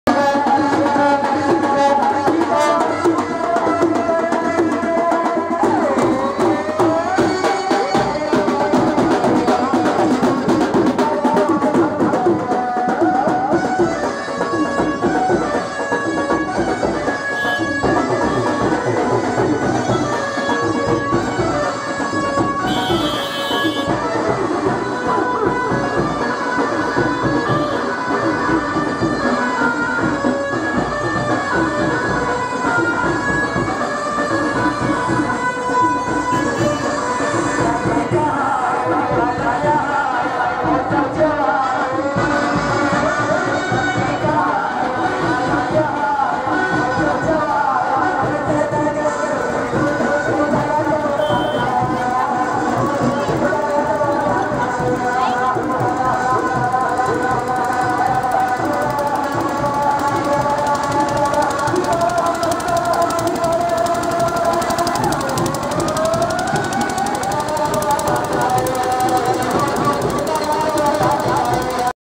Another Day in India
Marriage procession in the streets of Udaipur